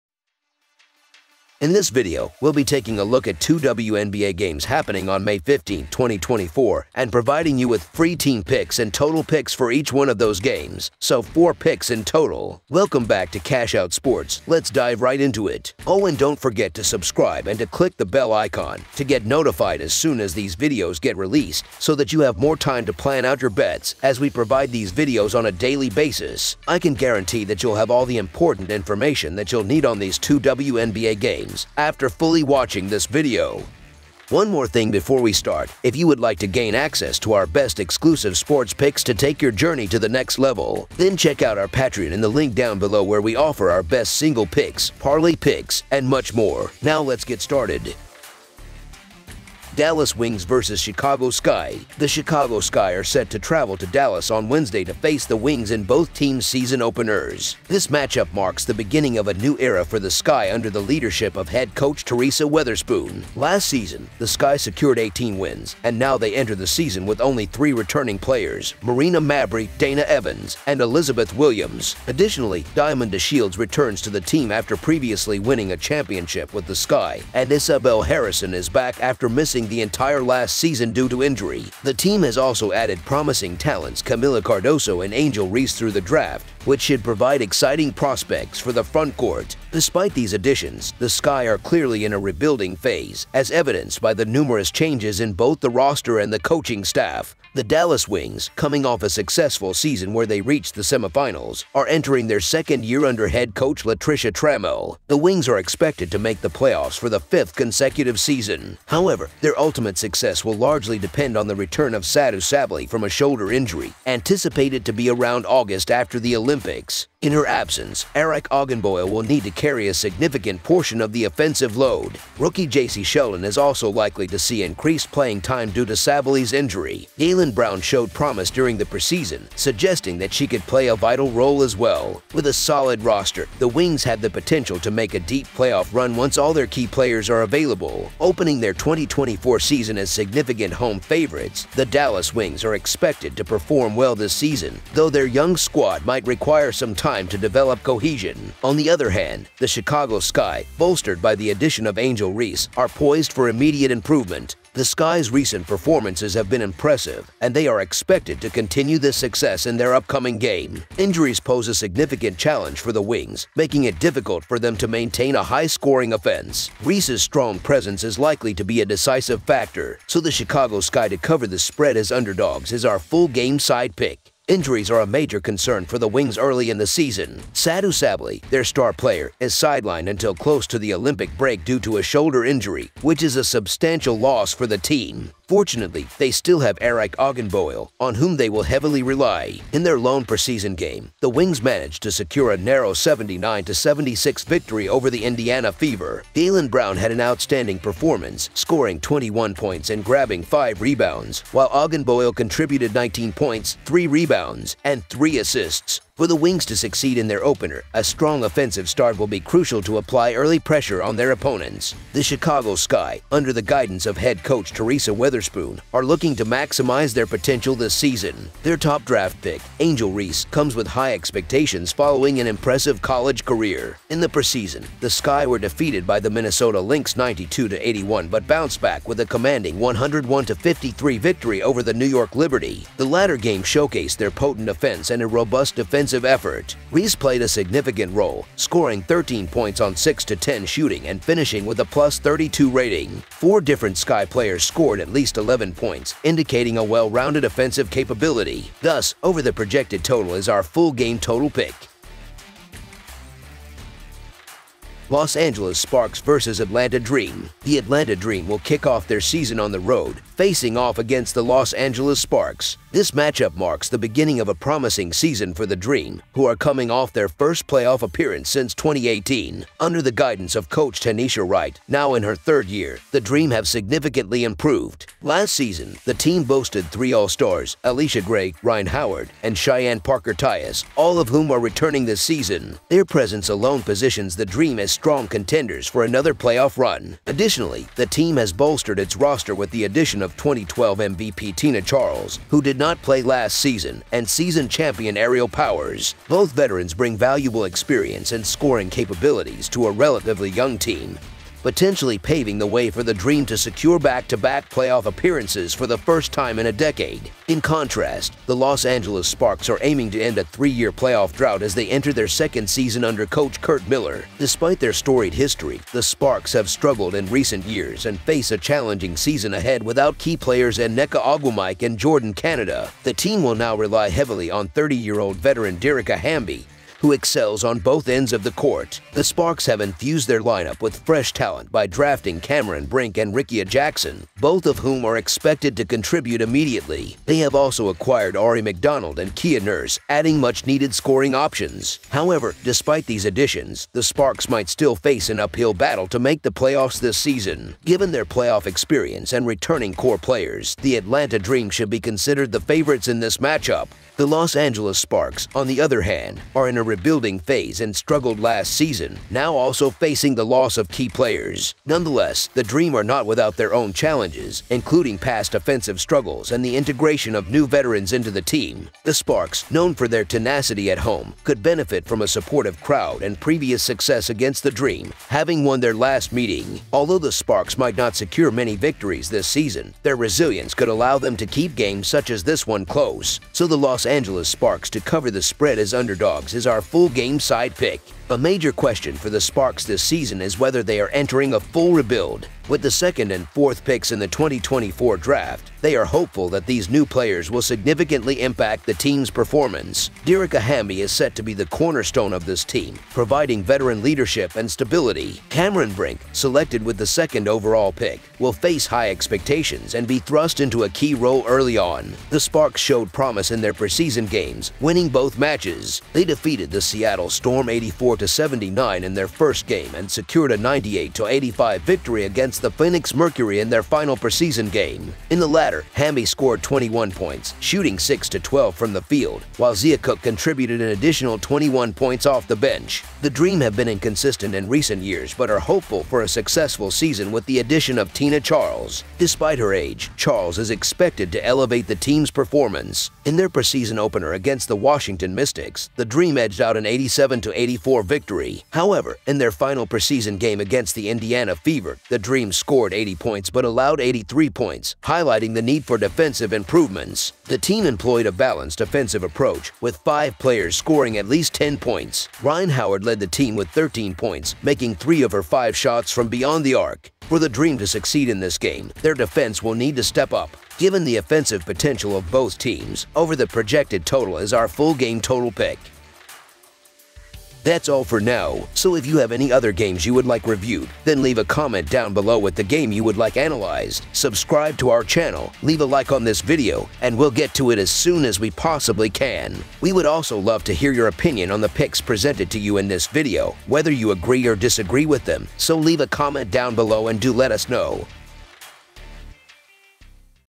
WNBA Picks Today 5/15/24 | WNBA Picks and Predictions Today 5/15/24
Dallas Wings vs. Chicago Sky
Los Angeles Sparks vs. Atlanta Dream

► If you would like to stay up-to-date with all our sports related content then join our free Telegram channel